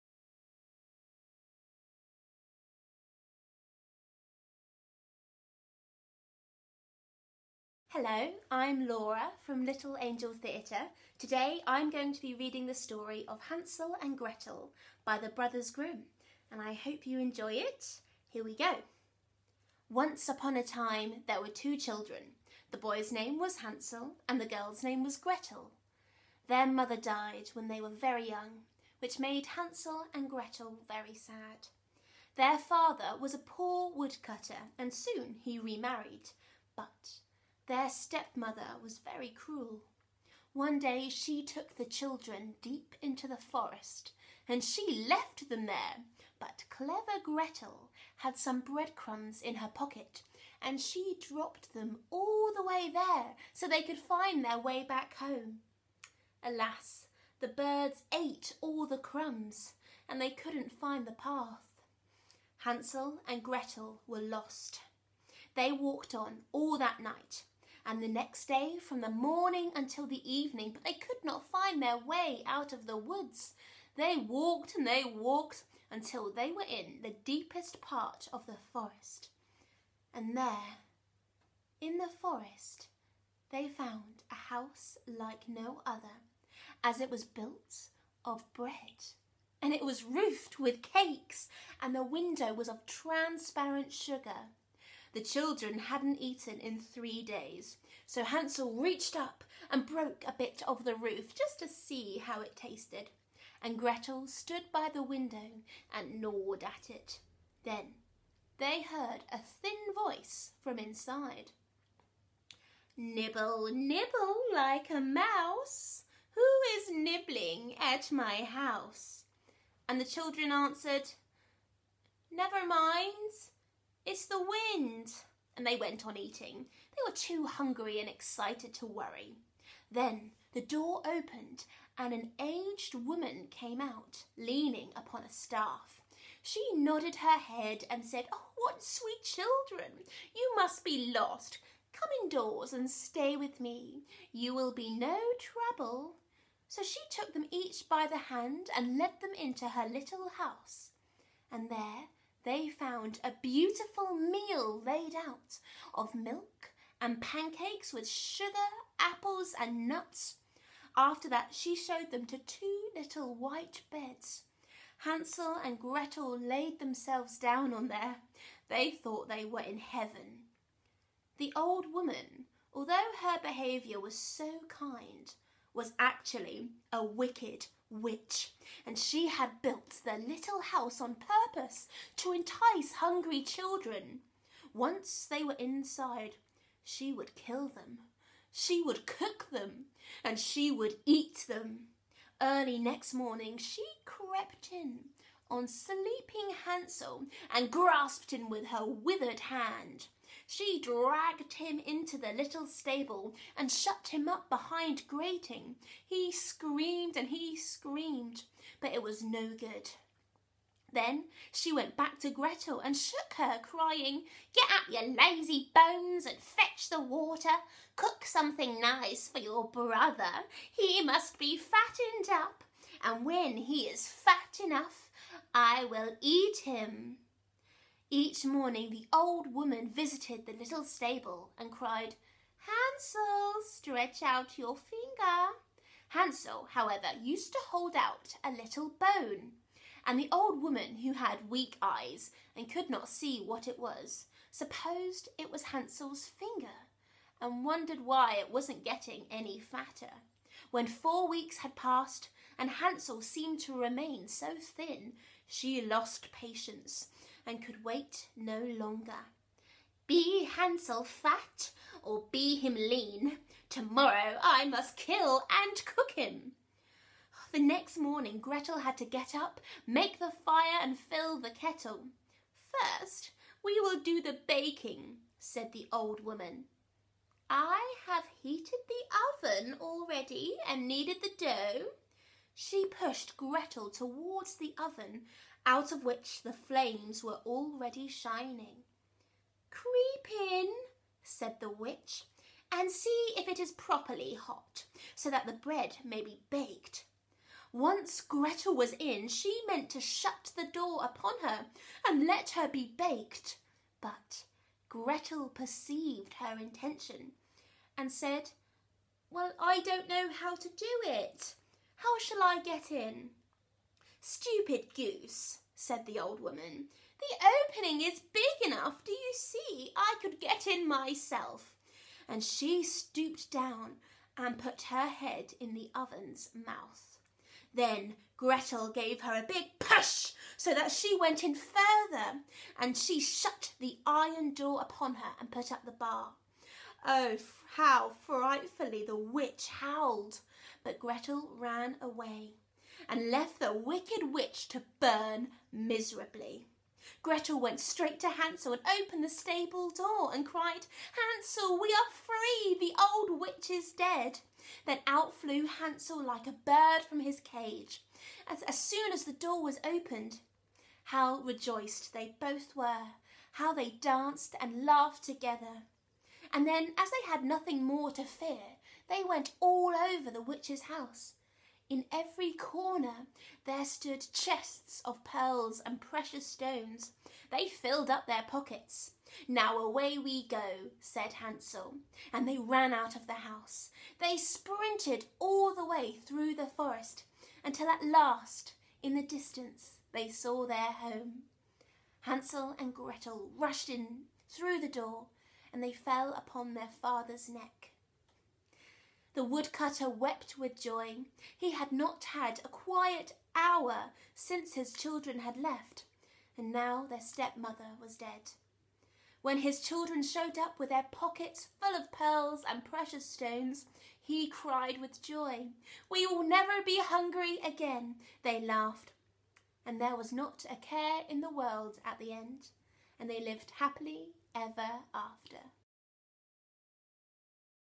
Hansel and Gretel I Stories for Children
Laura Doble reads 'Hansel and Gretel'.

This is a story for children in English.